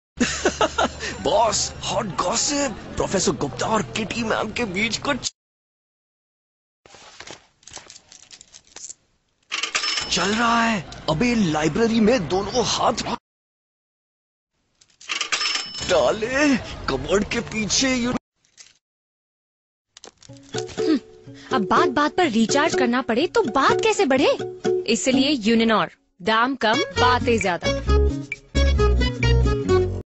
Uninor New Ad 2012 College."Pay less,talk more"
Uninor's New Ad of 2012 in College. "Pay less,talk more"